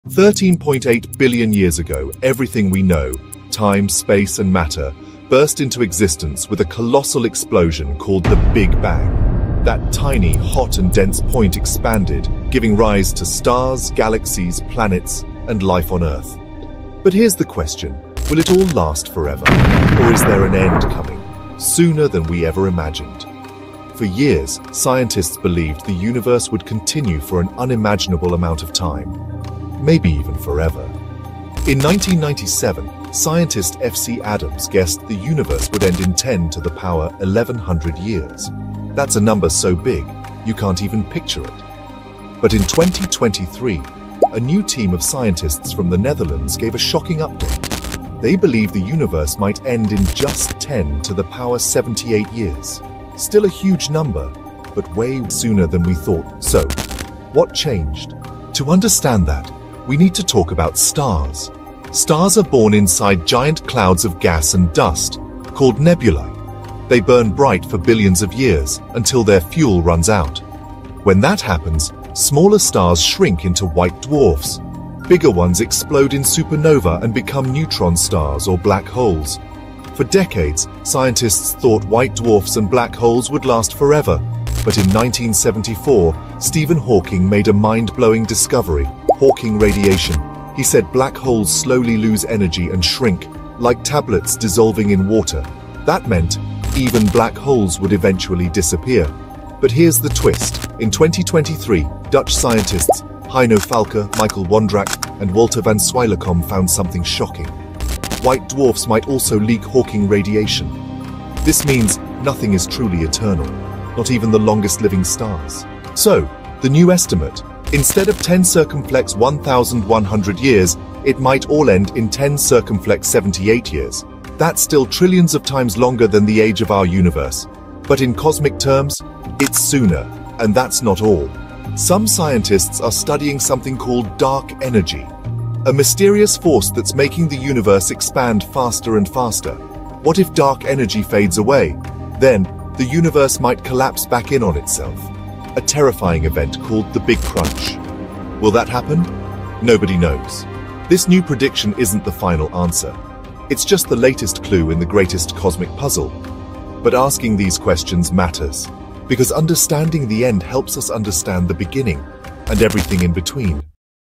Will the Universe End Sooner Than Expected? New Discovery Shocks Scientists!
What if everything — stars, galaxies, even black holes — disappears far sooner than we thought? In 2023, Dutch scientists revealed a shocking idea: the universe might die in "just" 10⁷⁸ years! Discover how Hawking radiation, black holes, and dark energy are rewriting our future in this cosmic story.

क्या सच में ब्रह्मांड 10⁷⁸ वर्षों में खत्म हो सकता है? जानिए कैसे white dwarfs और black holes भी Hawking radiation छोड़ सकते हैं और कैसे dark energy हमारे भविष्य को बदल सकती है।